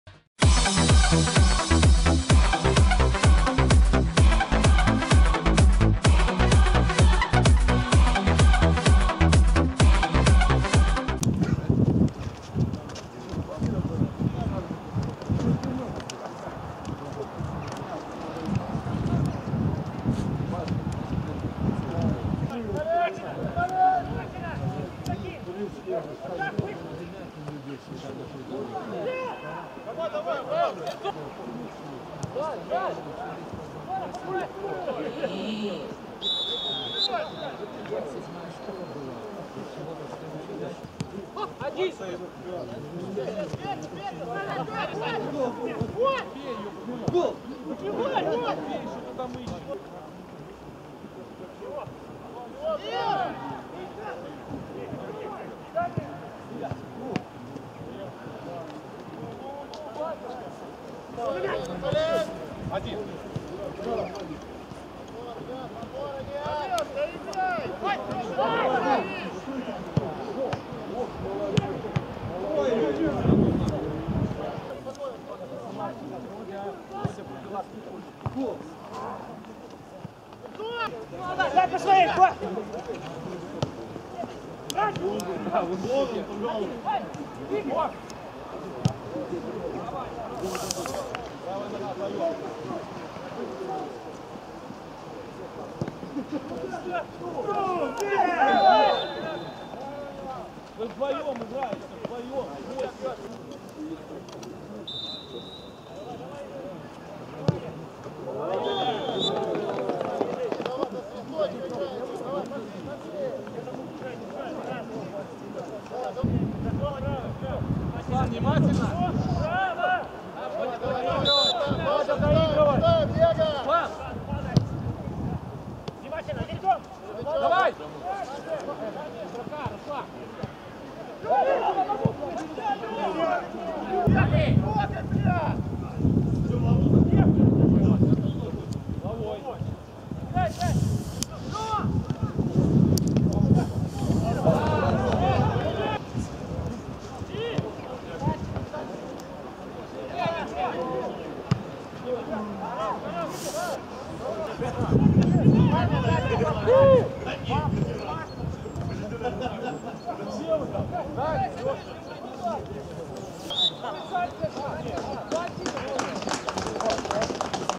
Гомель 1-1 Днепр. 17.02.2017
Видеообзор товарищеского матча Гомель 1-1 Днепр. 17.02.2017